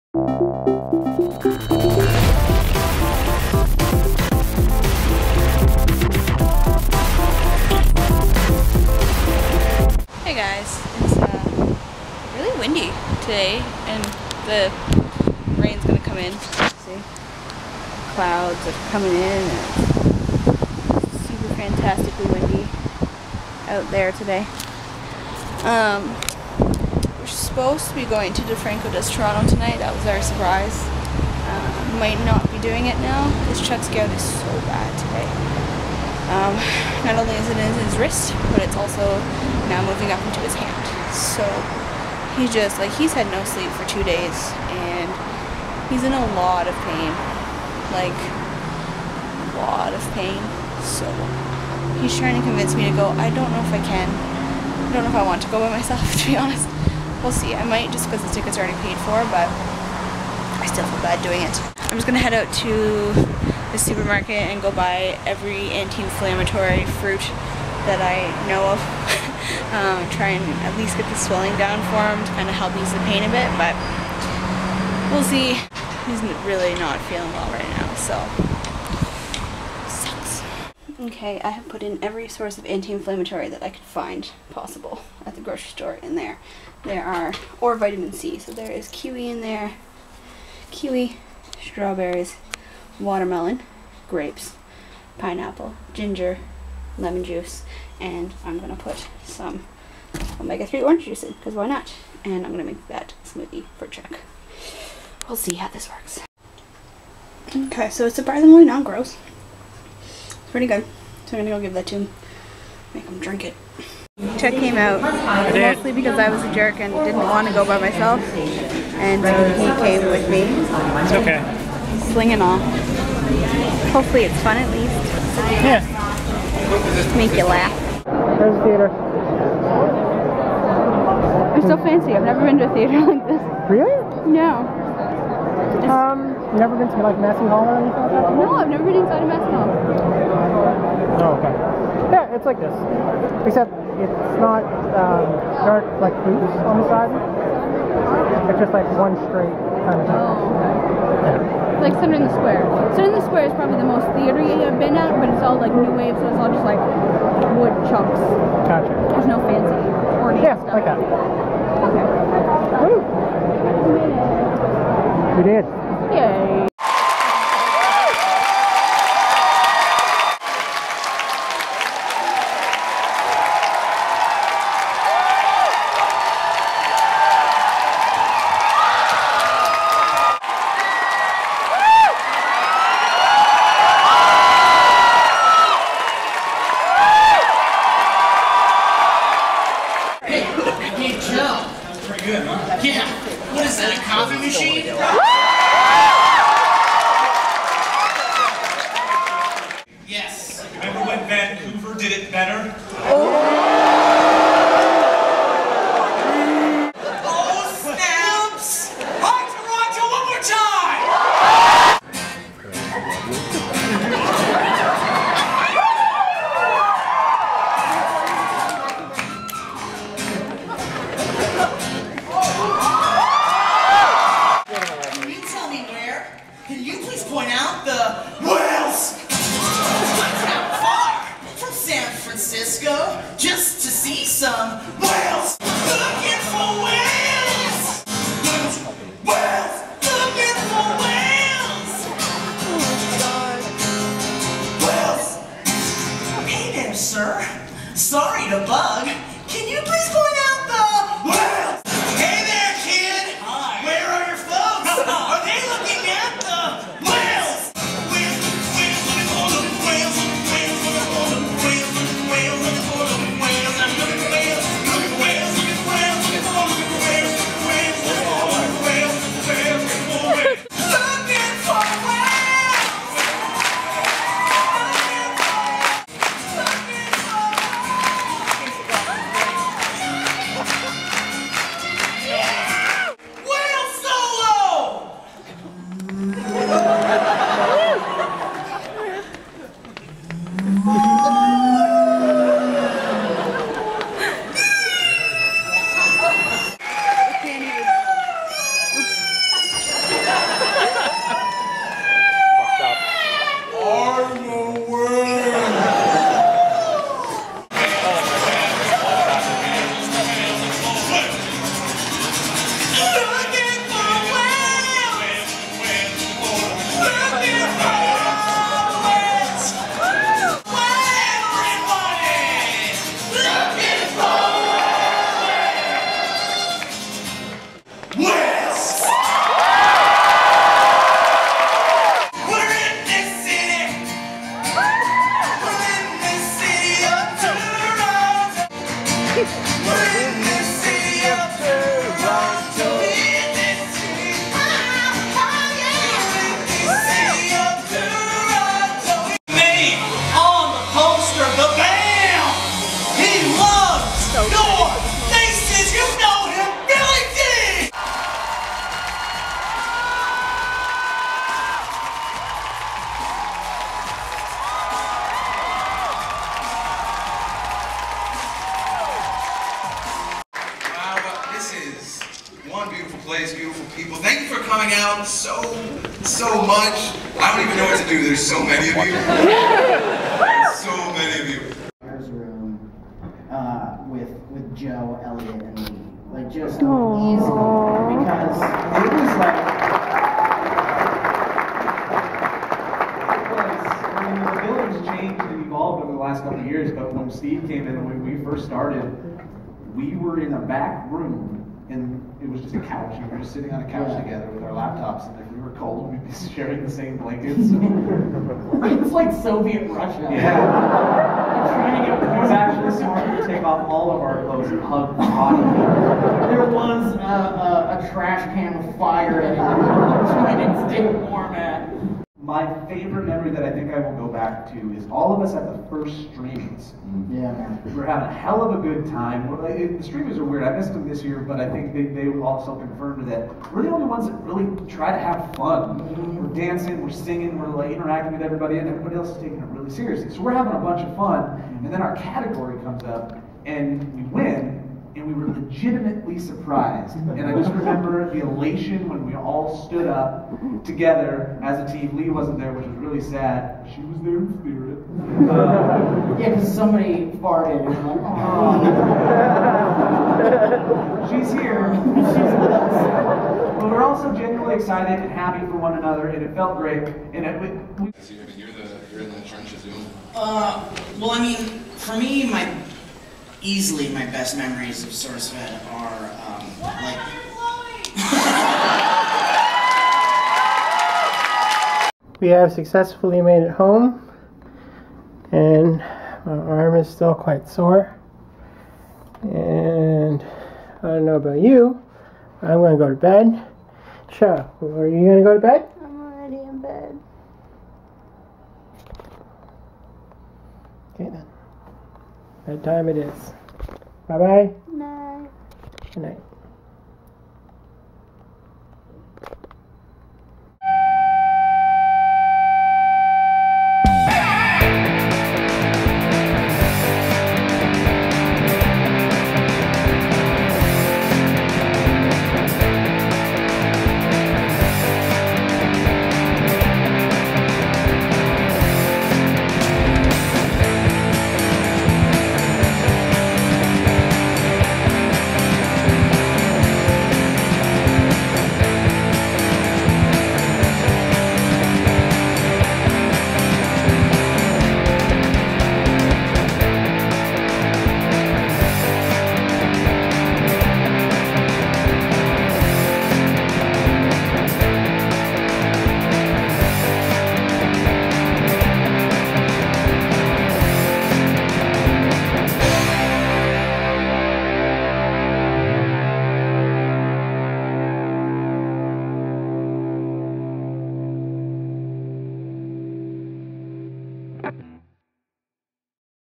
Day 478 - Pain, Fruit Smoothies and DeFranco Does Toronto!
Chuck is in a lot of pain, so Cheryl makes him an anti-inflammatory fruit smoothie.  They they brave it out, as things get weird at DeFranco Does Toronto!